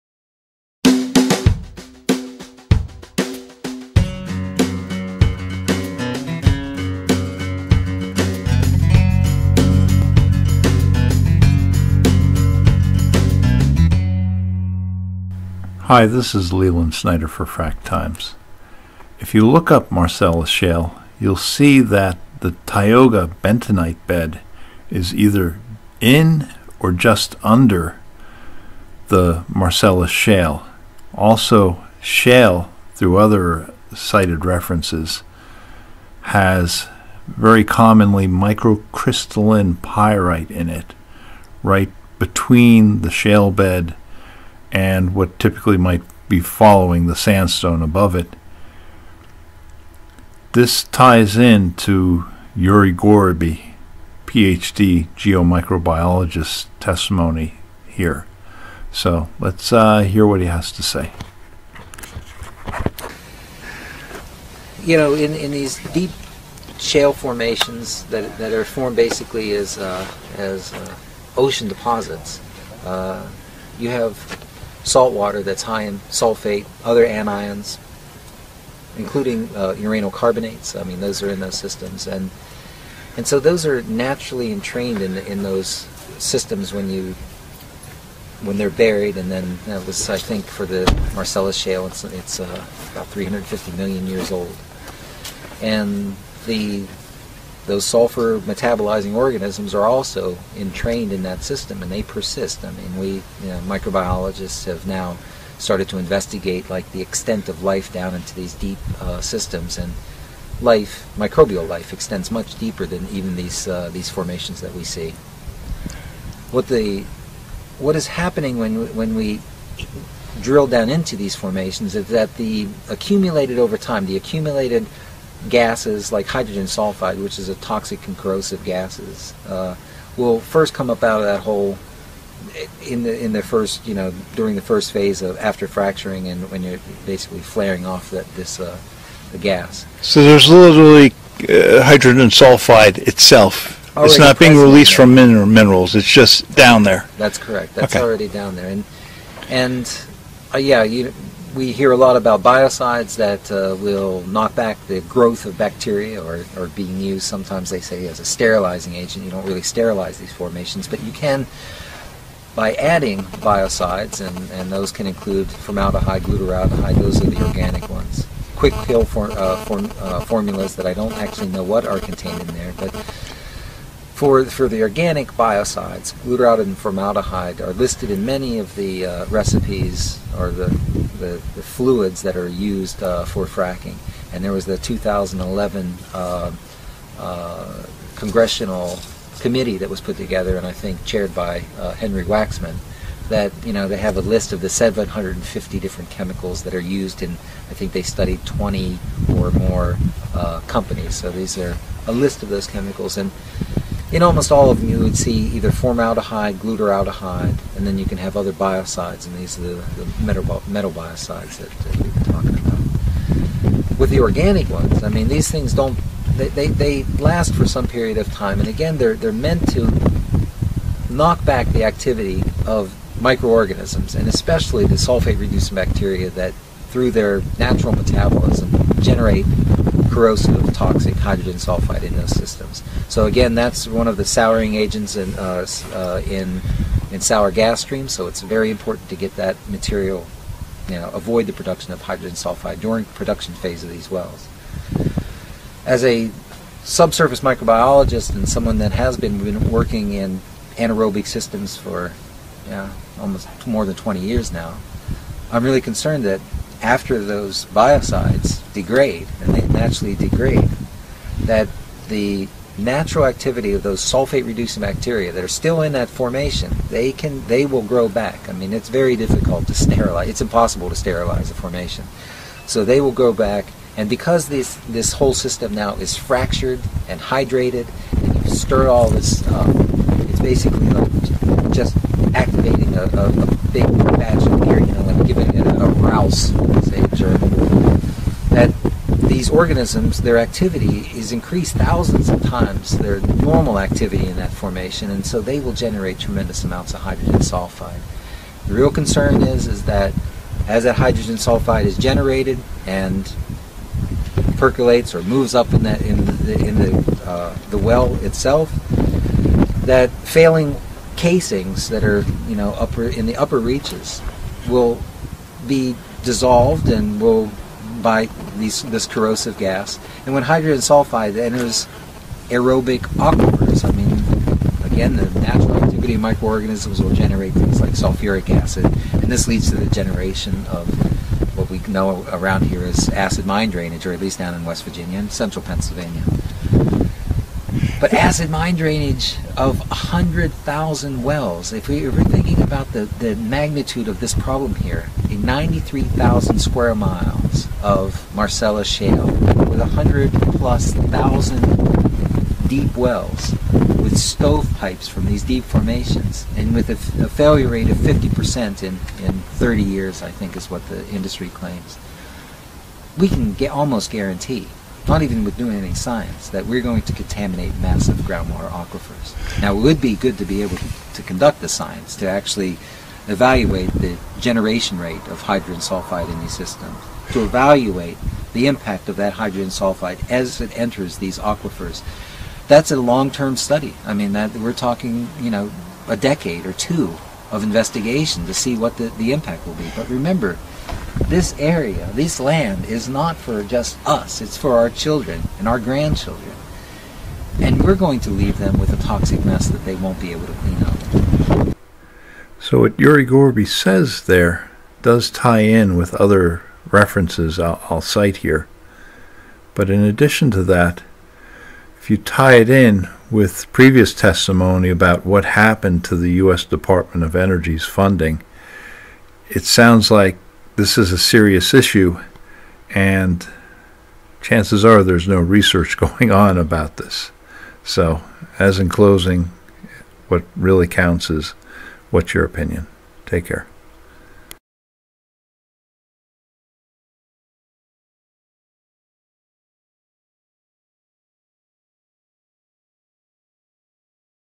Professor Yuri Gorby on Fracking Potential for AMD Acid Mine Drainage
Interview with Yuri Gorby on the potential of long term impact from anaerobic sulfur reducing bacteria as they relate to fracking and geobiology.
Taken at a area of ground water contamination.  To see the interview with the owner of the property describing issues, look up : "FROM THE FRONTLINES", Dave Bohlander This video is related to fracking, natural gas production energy, climate change, and pollution of our ground waters. The entire list of Pennsylvania water contaminations from fracking.